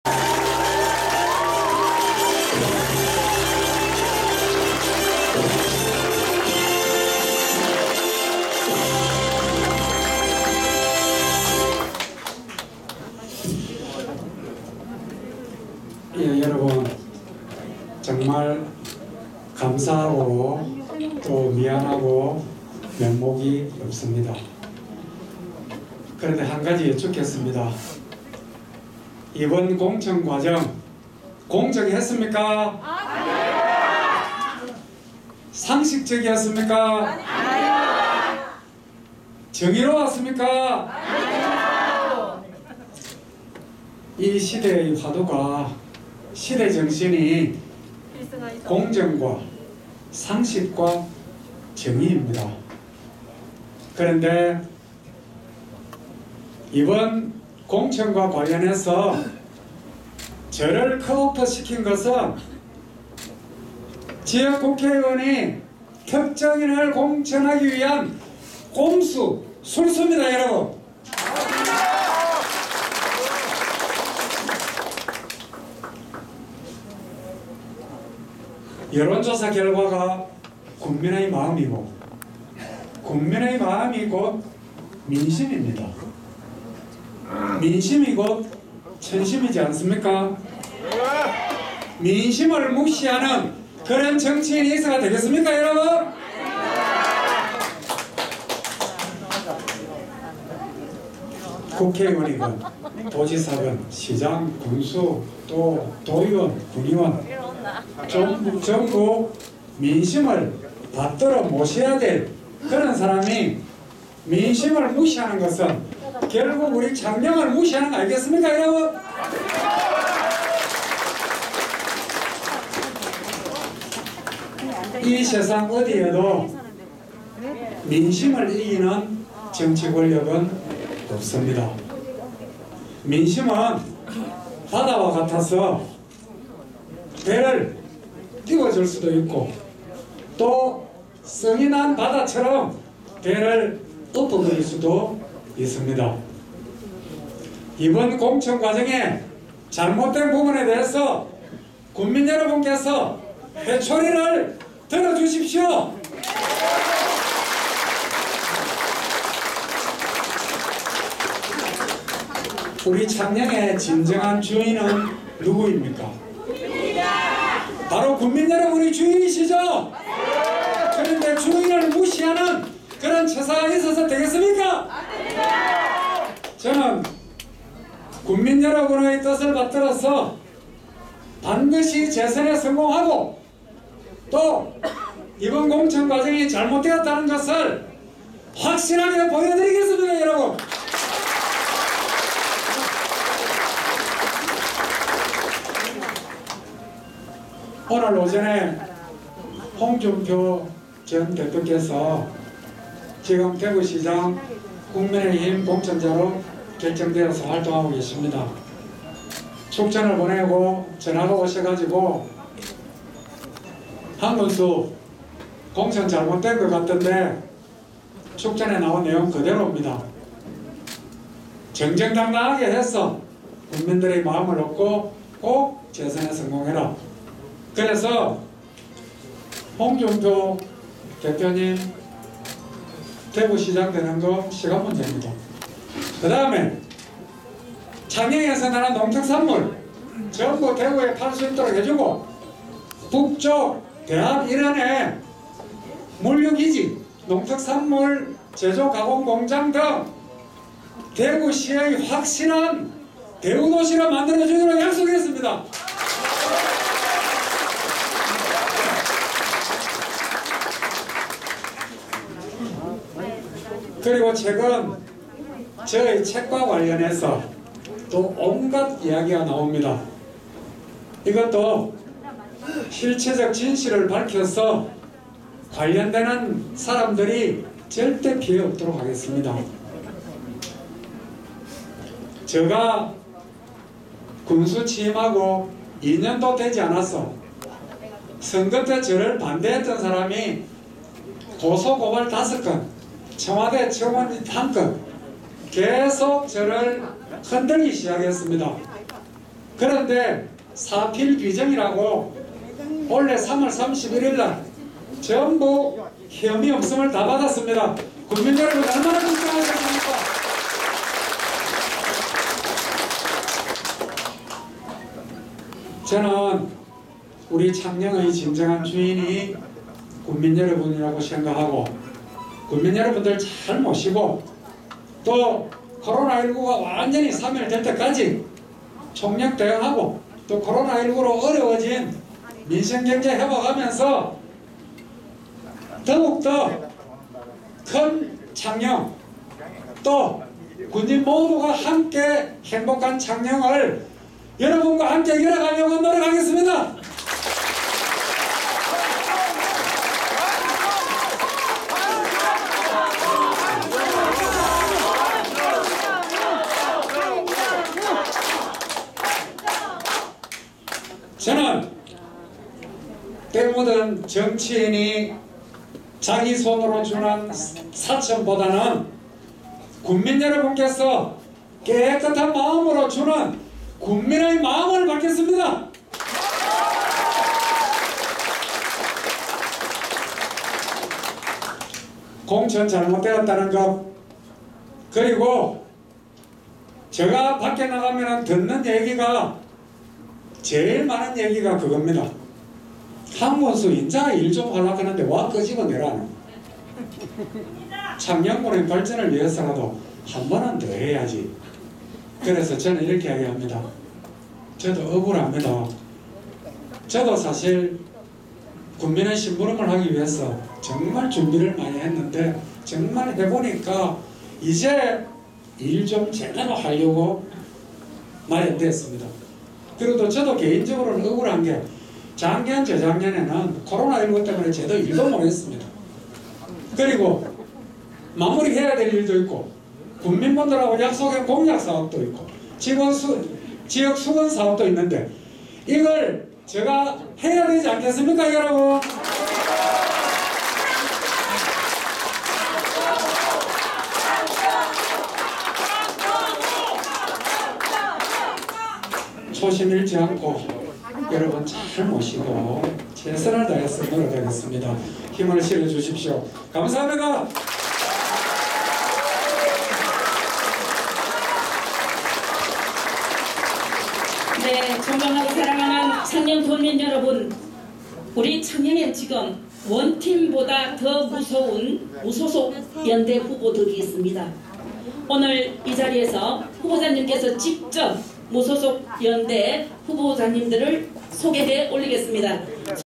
한정우 창녕군수후보 개소식 22.5.13
한정우 (전) 창녕군수 밀실 야합 공천 불만 무소속 출마
취재; 시사통신 경남취재본부